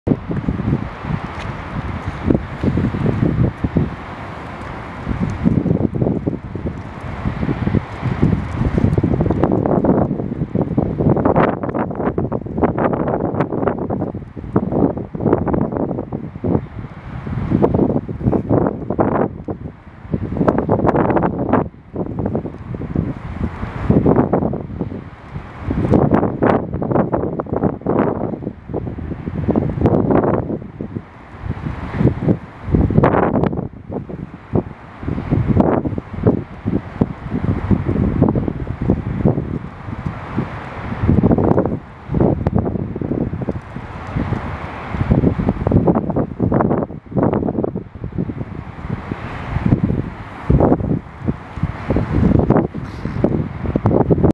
餌を探すハマヒバリ（Horned Lark）
手持ちの動画ゆえの水平撮りが後半破綻していますがお許しください